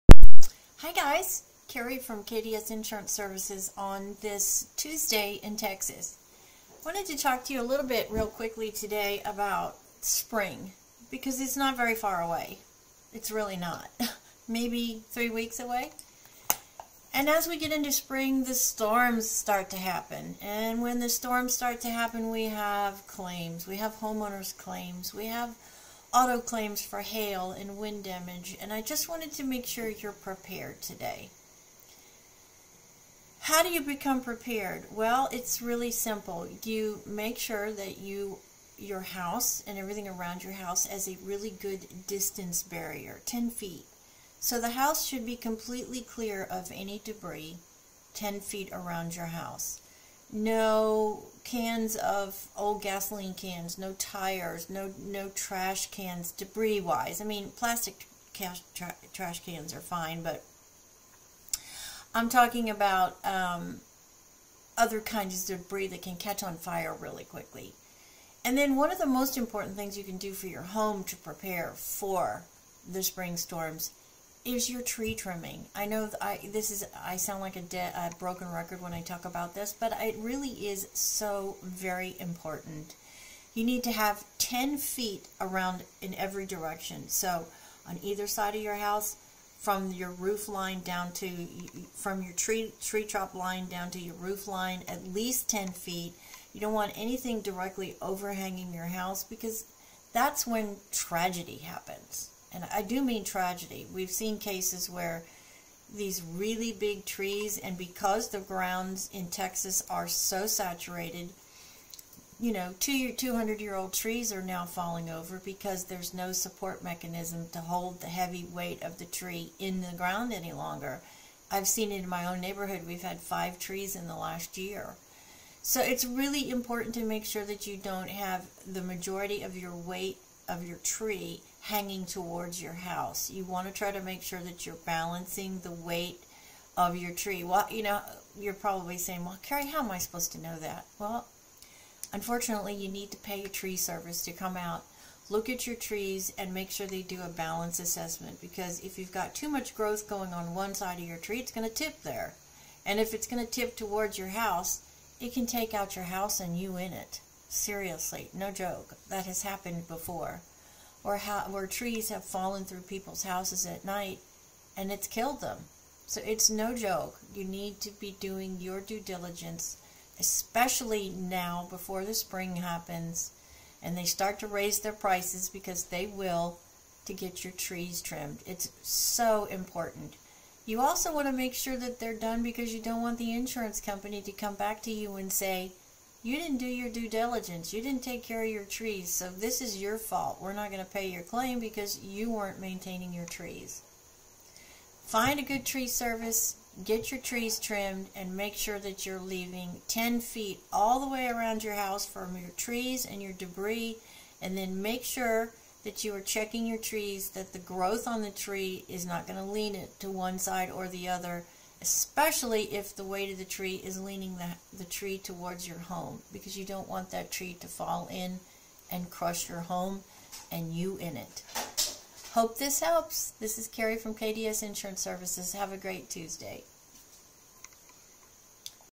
Spring storms are coming so are you ready?
Making sure you are prepared and your house is for the coming spring g storms and weather!